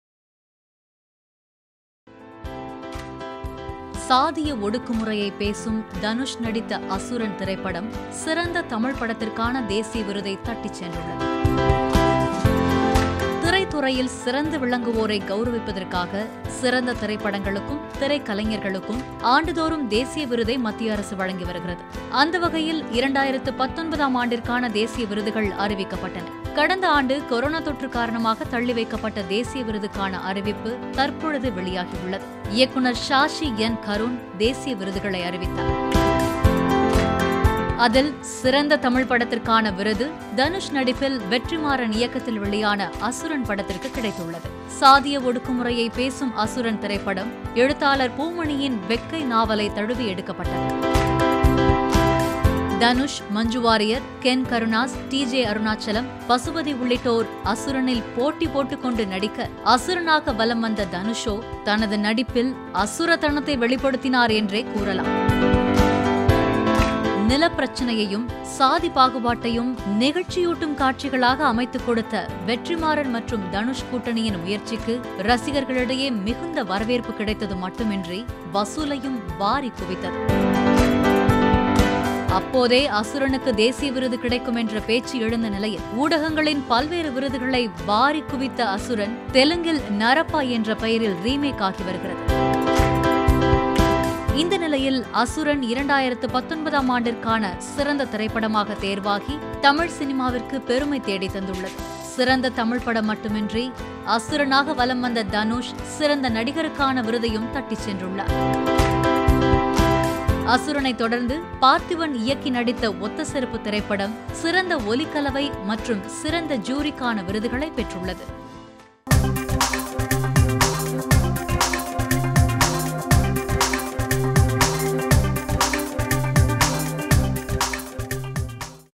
சாதிய பாகுபாட்டை பேசிய அசுரனுக்கு தேசிய விருது..!
அசுரனுக்கு தேசிய விருது..!

நடிப்பில் அசுரனான தனுஷுக்கு புதிய கவுரவம்..!